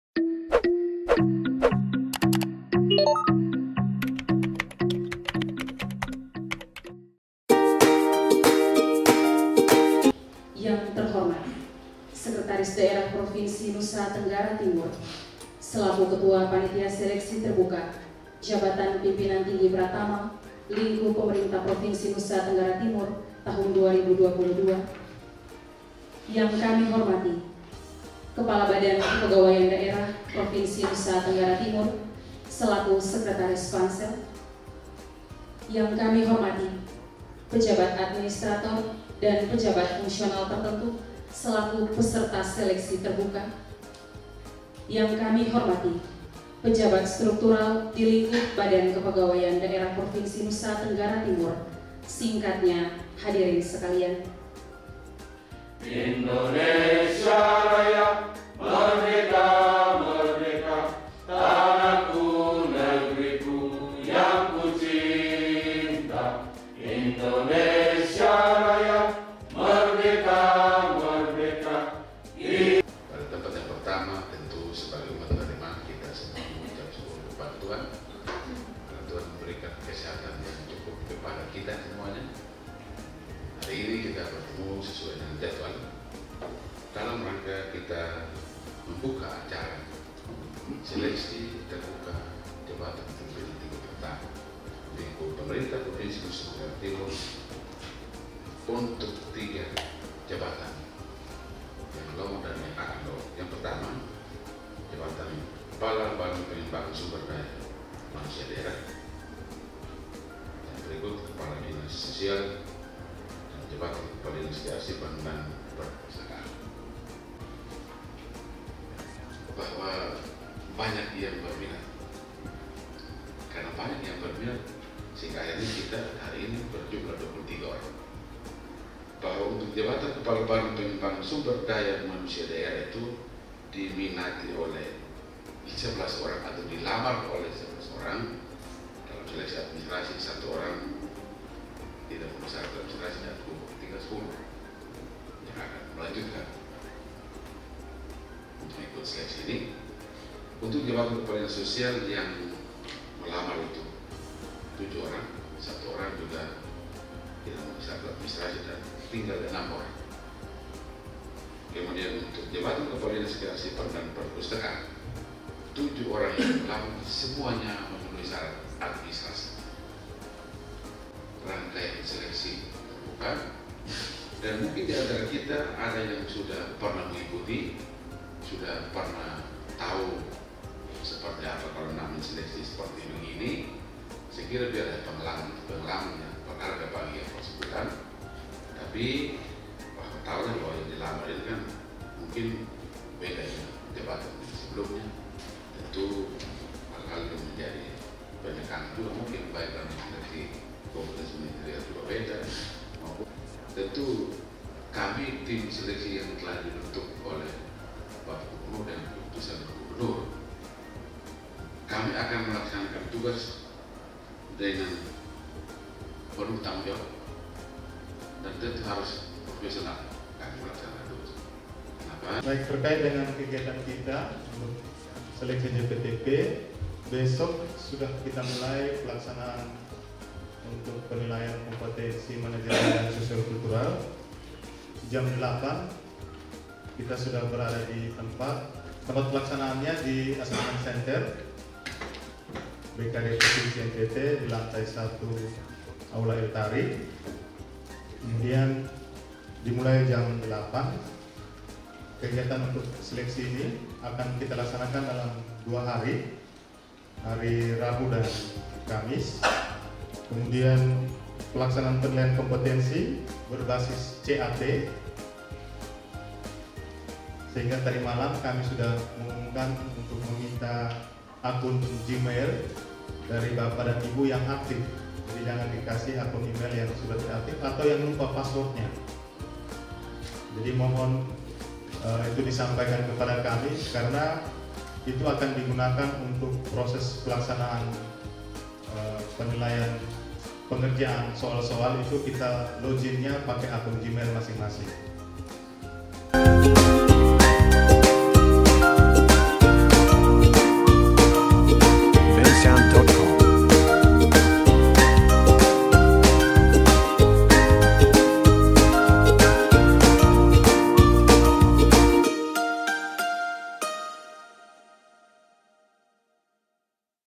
Part2: SELTER JPTP September 2022 - Acara Pembukaan | Badan Kepegawaian Daerah Provinsi NTT
Badan Kepegawaian Daerah Provinsi NTT dalam hal ini Assessment Center Provinsi NTT melaksanakan Seleksi Terbuka bagi 3 Jabatan Pimpinan Tinggi Pratama (JPTP) yang lowong di Lingkup Pemerintah Provinsi NTT.

Videographer & Editor :

      Periantu M. Sabuna